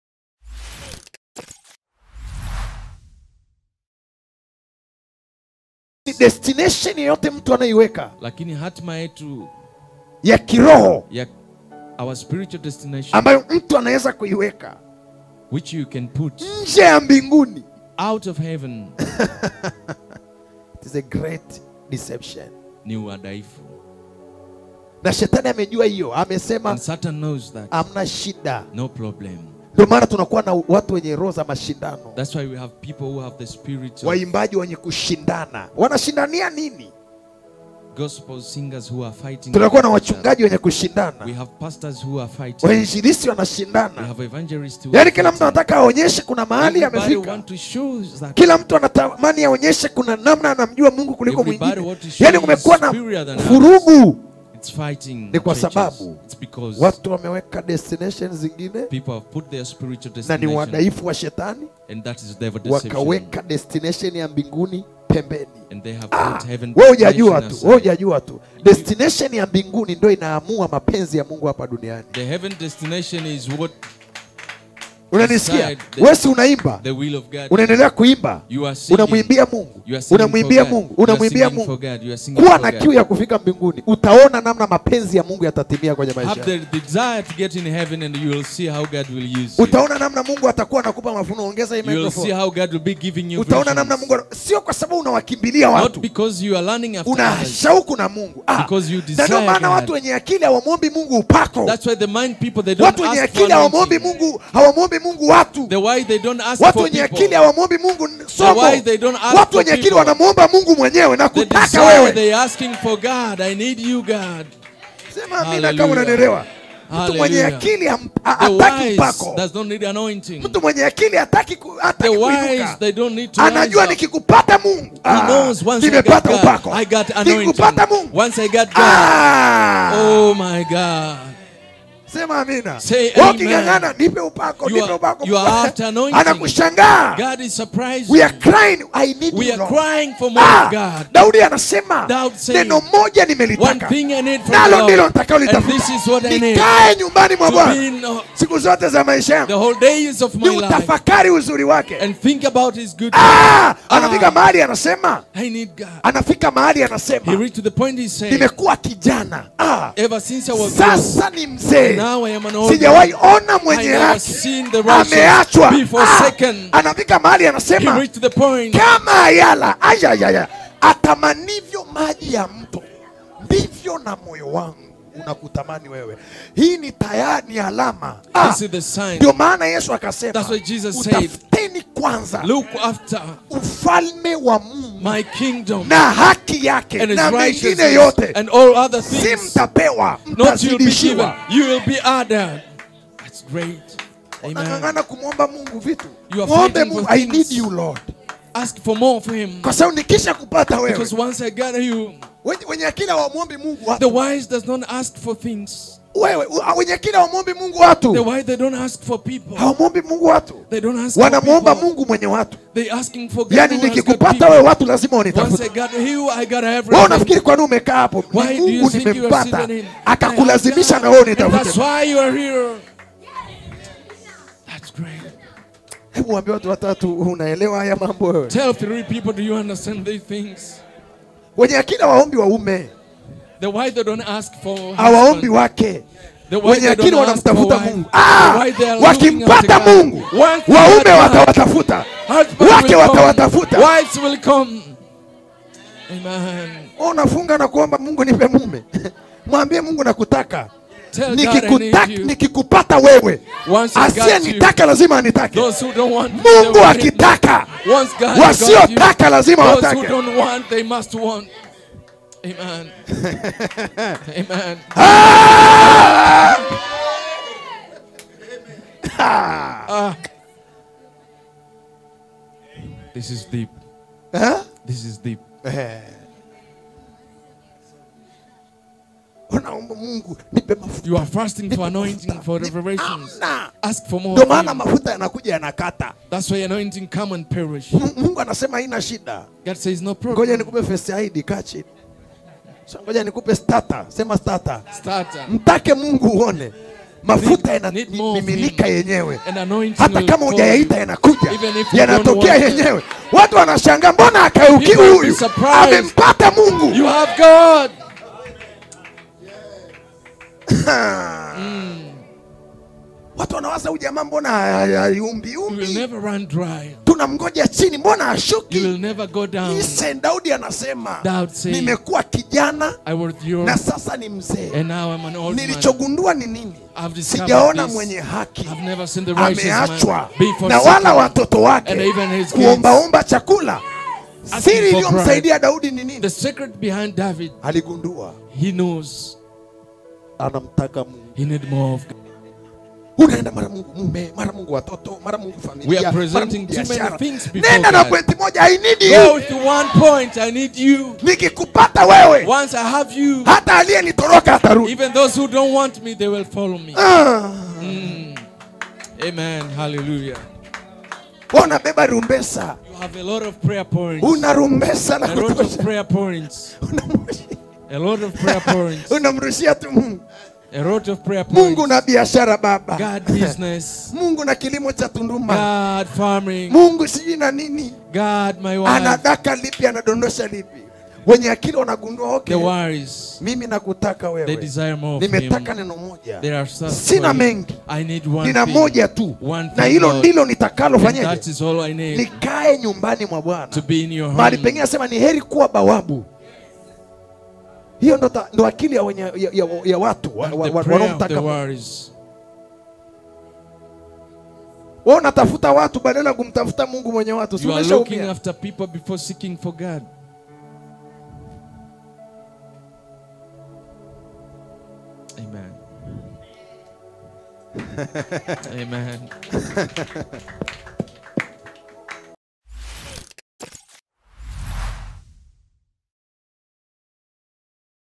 HATIMA YAKO IHUSIANE SHARTI MBINGUNI | EV. DANIELY JOSHUAN
Hii ni sehemu ya somo la "MAFUNUO 18 JUU YA SHETANI" liliofundishwa na mtumishi wa Mungu Ev. Daniely Joshuan. Ikiwa hukupata neema ya kusikiliza somo nzima bofya link hii